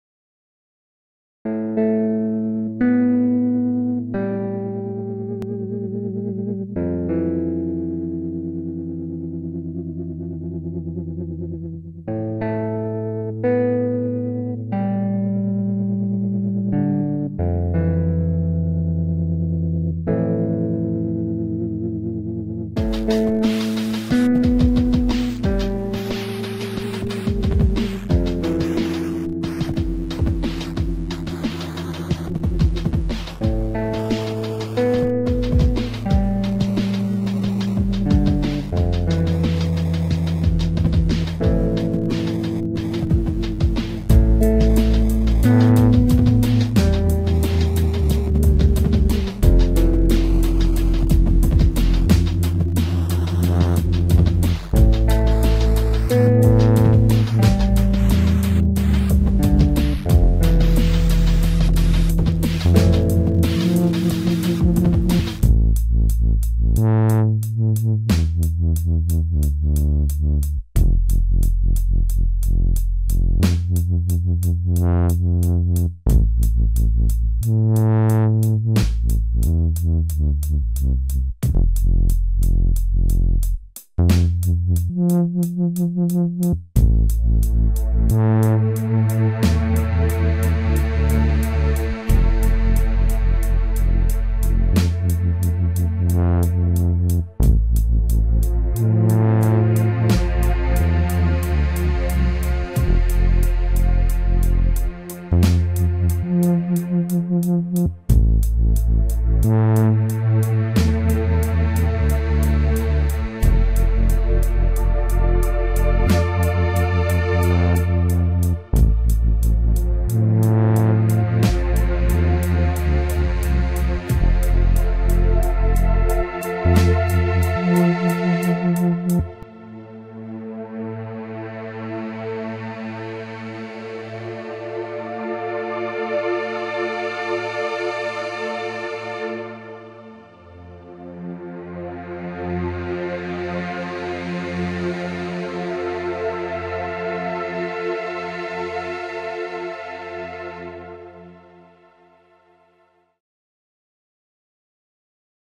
UDO Super Six all parts
trying to show off the UDO Super Six's versatility. drums in Ableton, everything else the S6. still running through the presets.